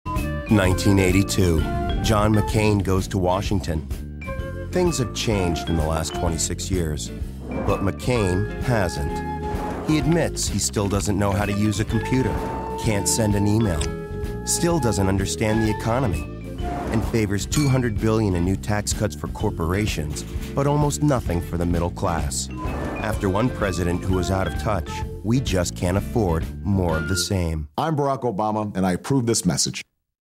Obama ad: McCain's out of touch
Source: CNN

A new Obama ad paints McCain as out of touch and unable to implement change.

My verdict: While McCain goes for the outright false and negative ads, Obama goes for the humorous and truthful ads.

McCain still lives in the 60's. America cannot elect this man as President